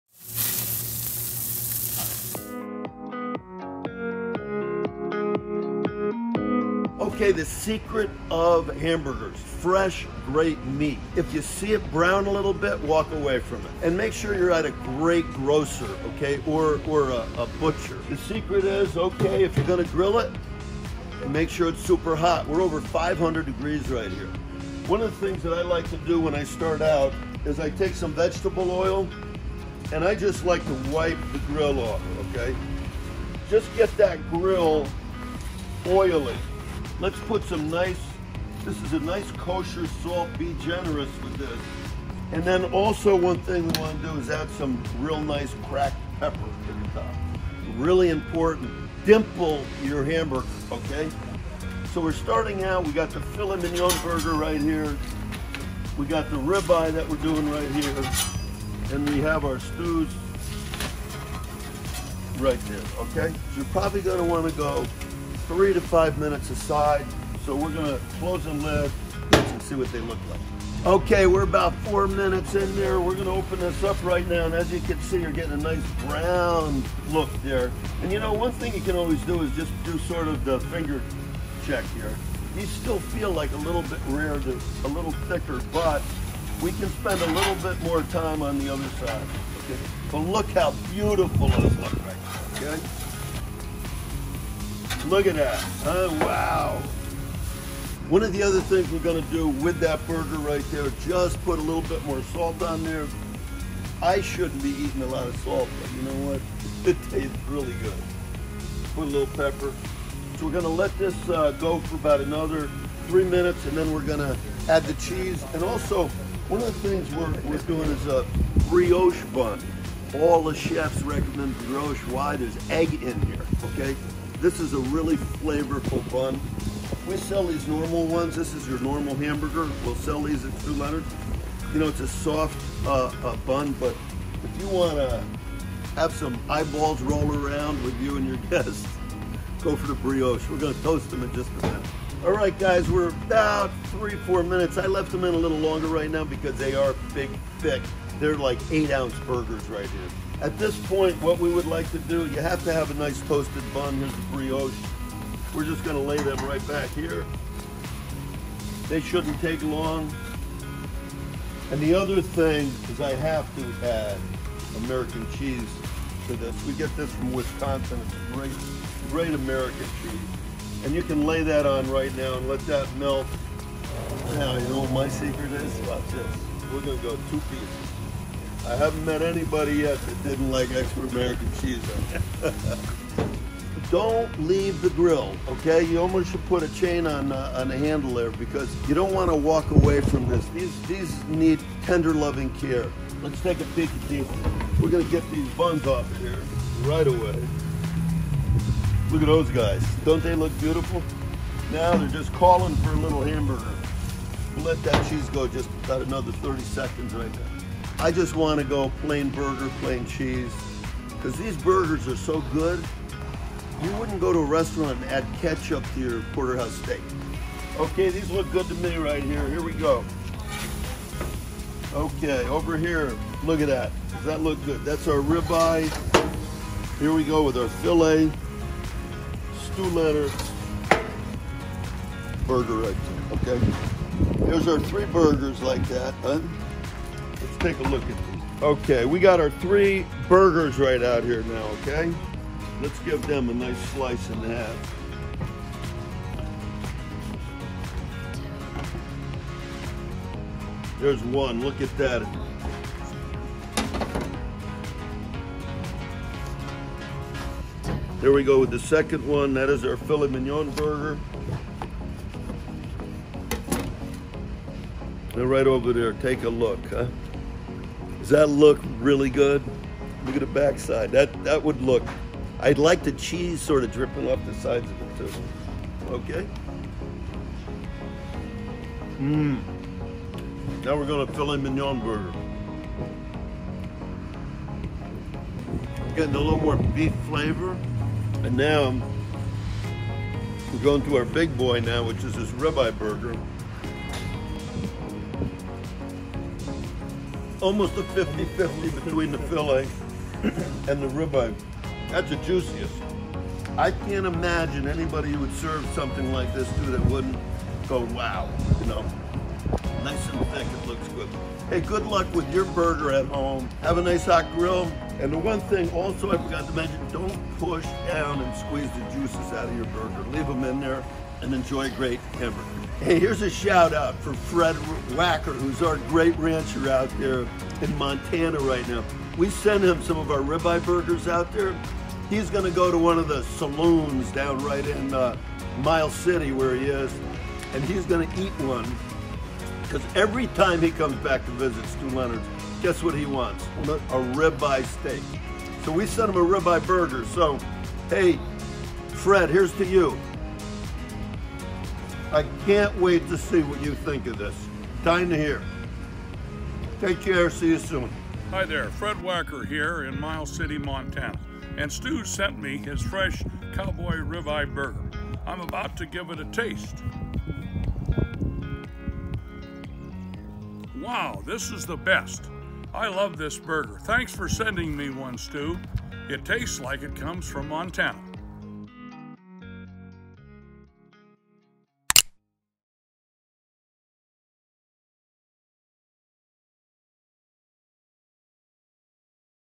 Stew Grills Gourmet Cheeseburgers
The best cheeseburger starts with good quality beef and a super simple recipe for grilling. Here, Stew grills up 3 different cheeseburgers using delicious ribeye, filet mignon, and ground chuck patties. Watch to find out which is his favorite.